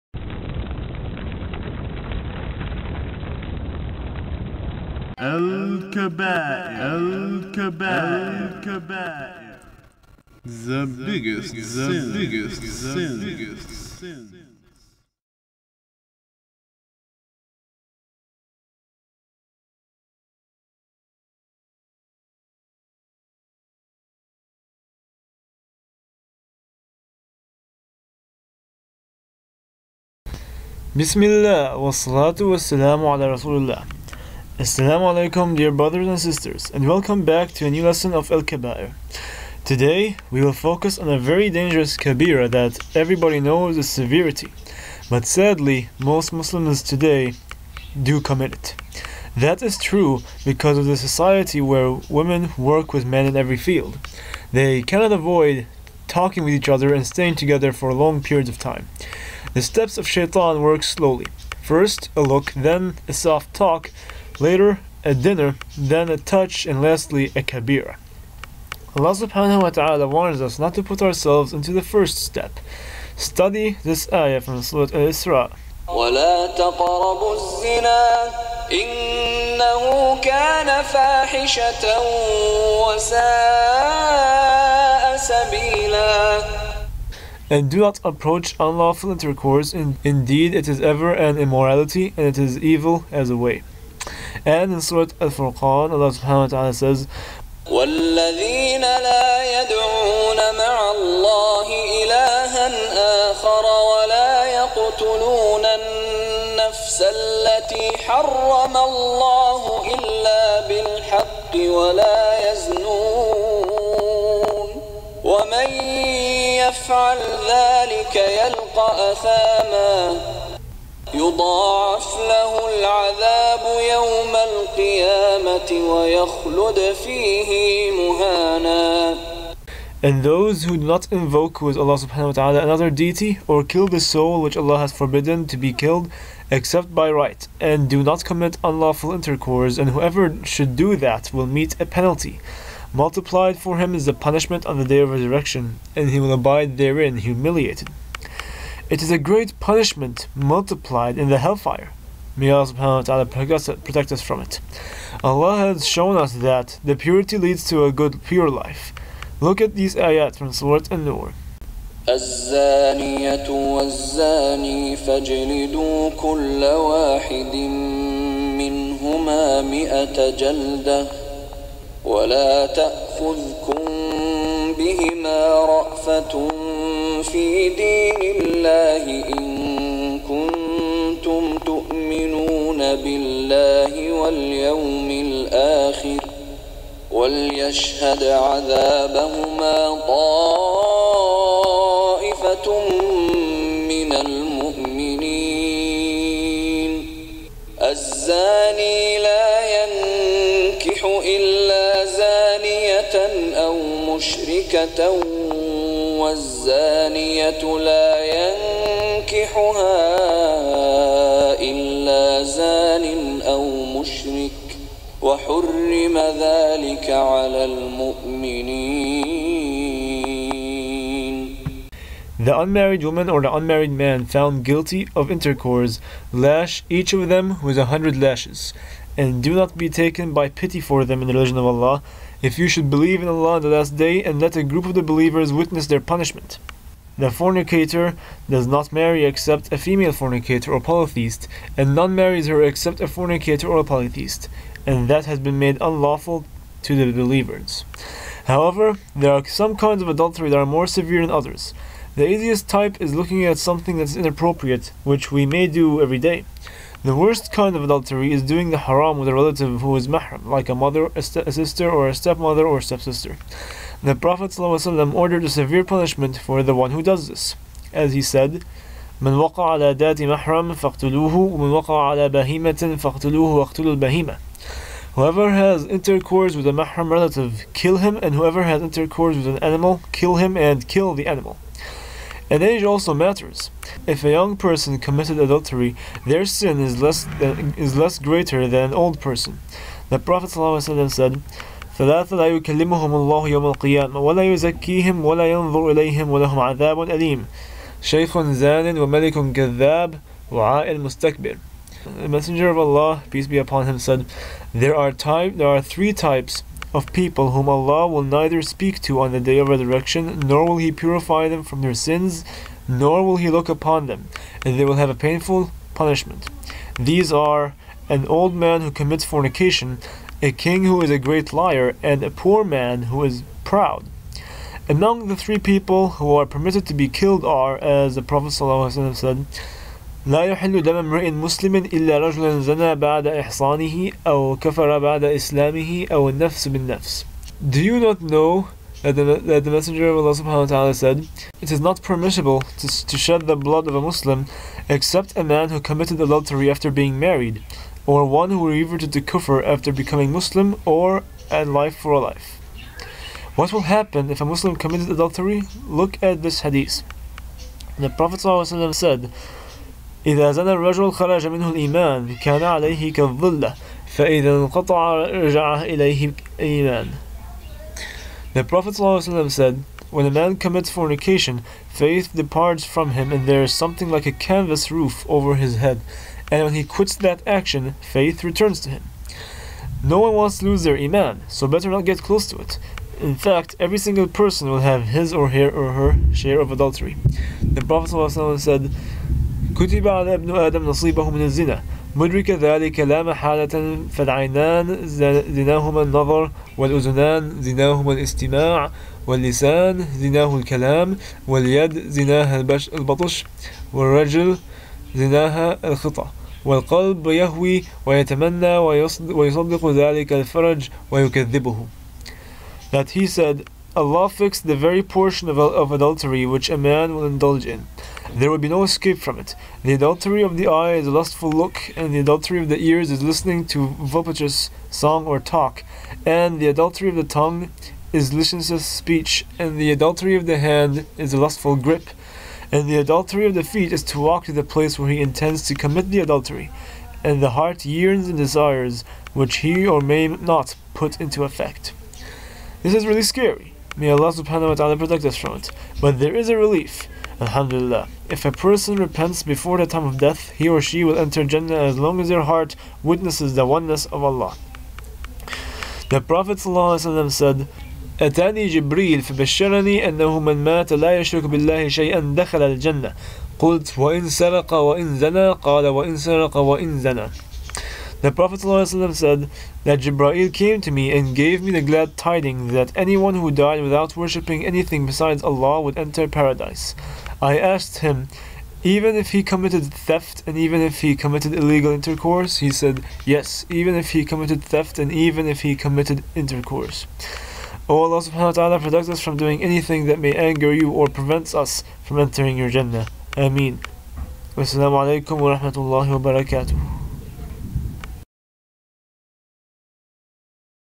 The Greatest Sins: 9 Adultery
The greatest Sins is a series of short lessons that teach Muslims about the worst sins in order for us to refrain from this kabeera and win the Jannah. Adultery is a shameful sin that a Muslim must take in consideration and stay away from.